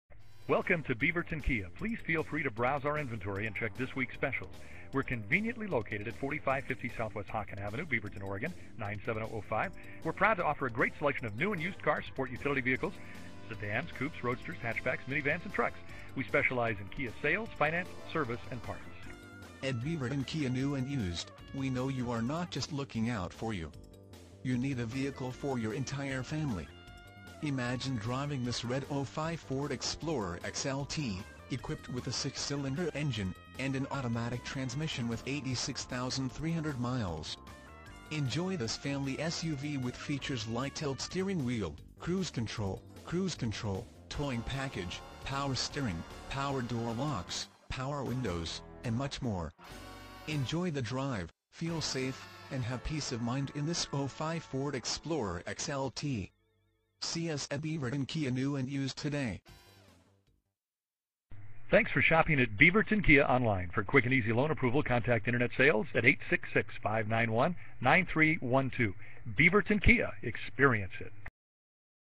2005 Ford Explorer XLT Beaverton Kia New/Used Beaverton,
2005 Ford Explorer XLT- At Beaverton Kia New/Used, we know you are not just looking out for you. You need a vehicle for your entire family. Check out this Red 2005 Ford Explorer XLT, equipped with a 6 Cyl. engine and an automatic transmission with 86,300 miles. Enjoy this family SUV with features like Tilt Steering Wheel, Cruise Control, Cruise Control, Towing Package, Power Steering, Power Door Locks, Power Windows, and much more. Enjoy the drive, feel safe, and have peace of mind in this 2005 Ford Explorer XLT. See us at Beaverton Kia New/Used today!
Miles-  86,300
VIN- 1FMZU73E05ZA31307

Beaverton Kia New/Used
4450 SW Hocken ST.